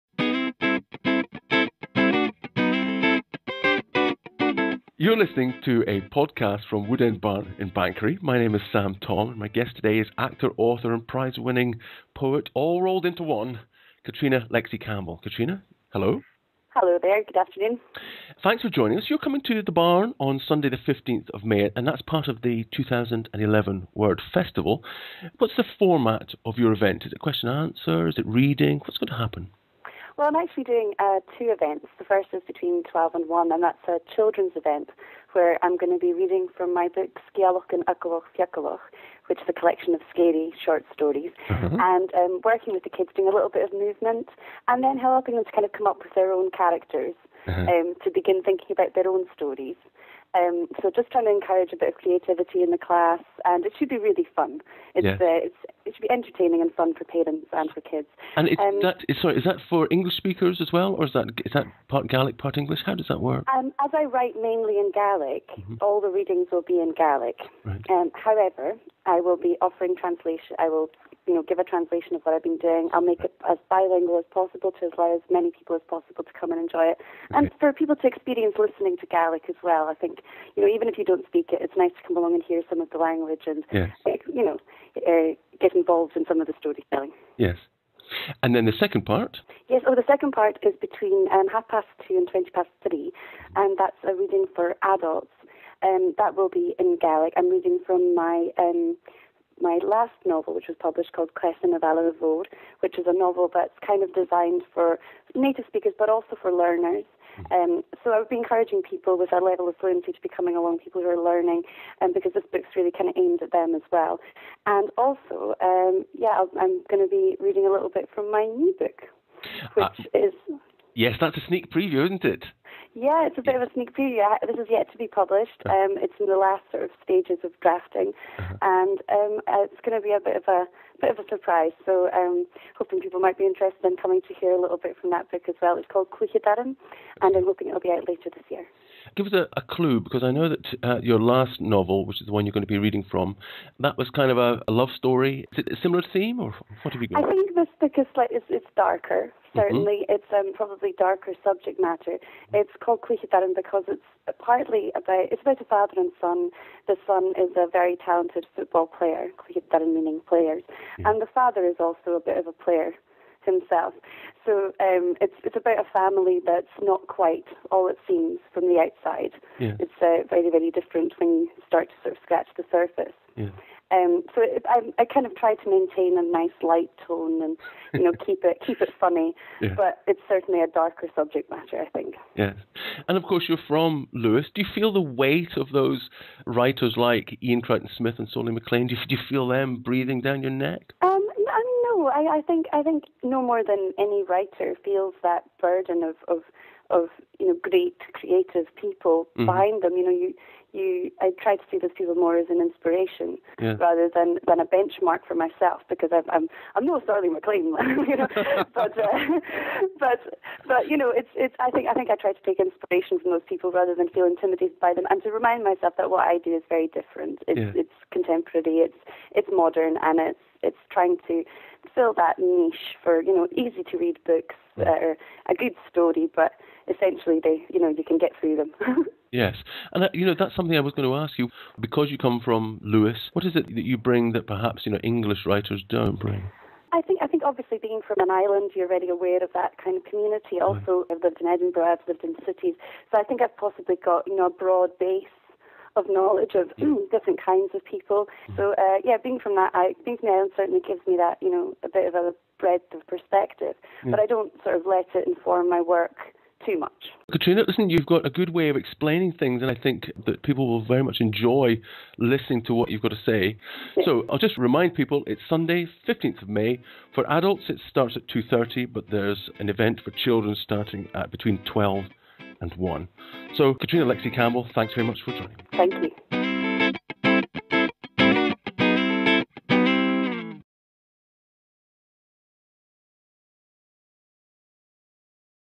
Woodend Barn: Catriona Lexy Cambell & Word 2011
Author, actress and poet Catriona Lexy Campbell discusses being a Gaelic writer as she prepares for her appearance at The Barn as part of Word 2011 literary festival.